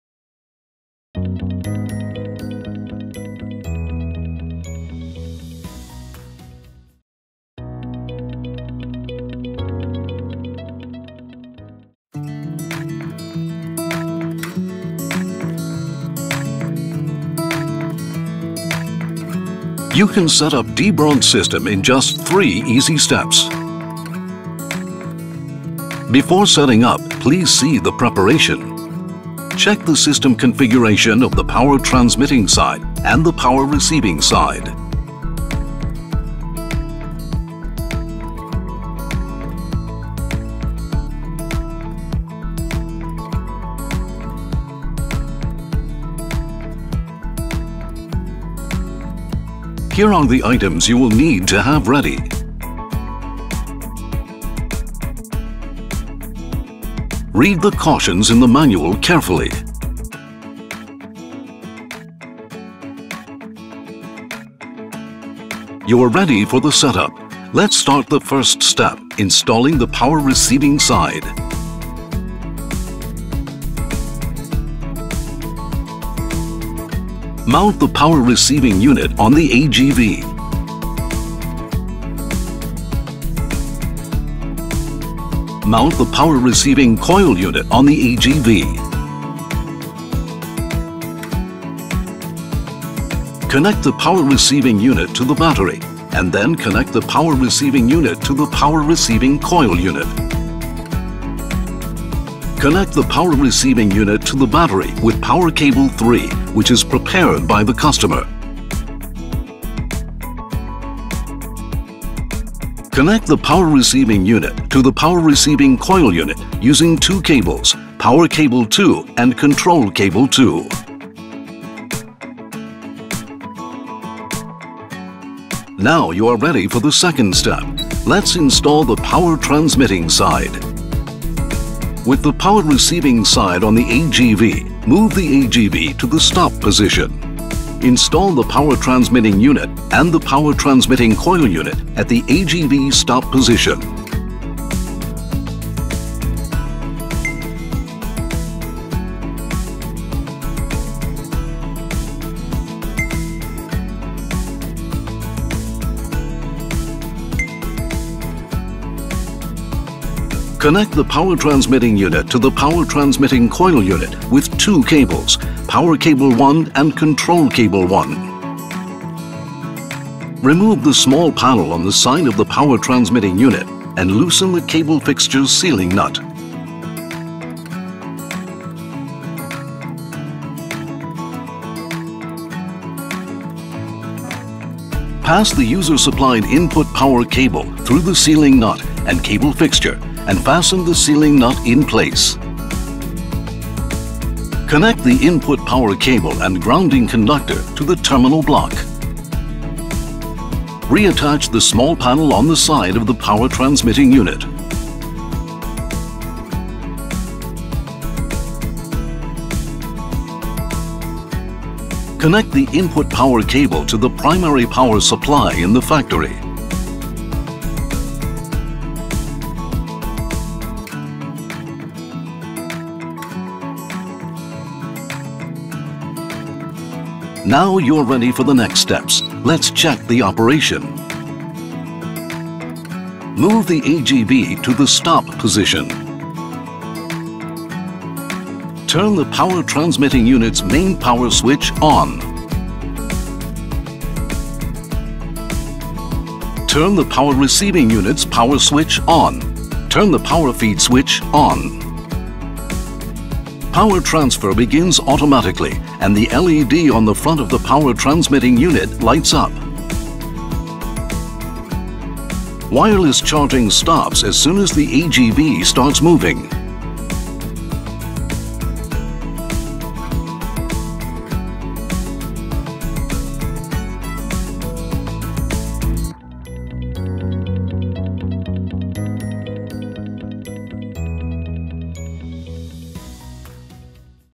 BeeWaTec Autonome Mobile Roboter | Kontaktlose Energieübertragung mit BEECHARGE
Das BEECHARGE-System besteht aus 4 Einheiten und ermöglicht einen konstanten FTS-Betrieb über 24 Stunden.